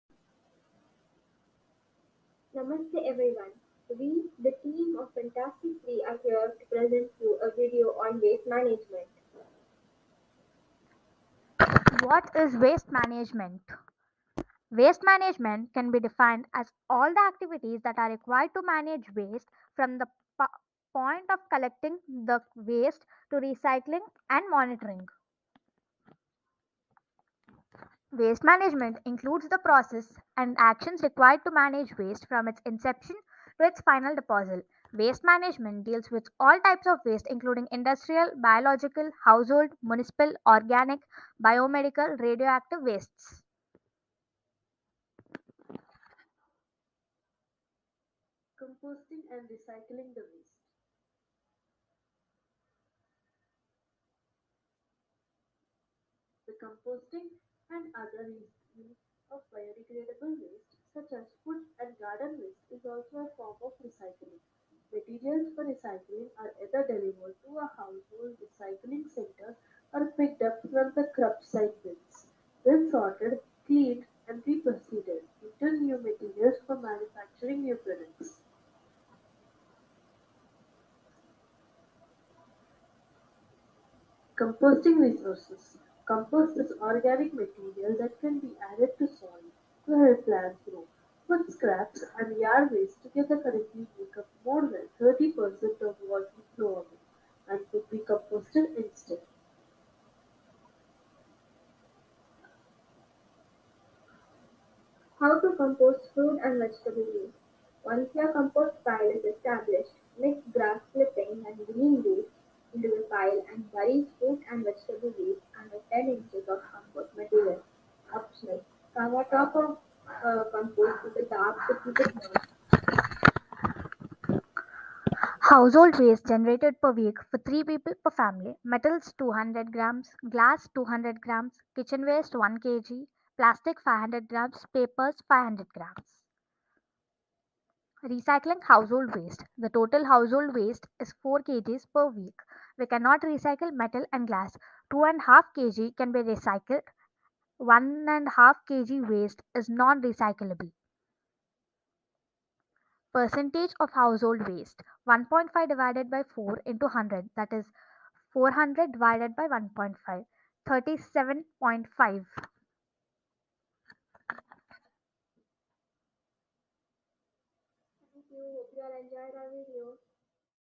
Fantastic 3 Waste Management
This video is made by a team participating in Up-Tix ideafest 2022 - National inter-school team competition on Sustainability.

for more such exciting competitions.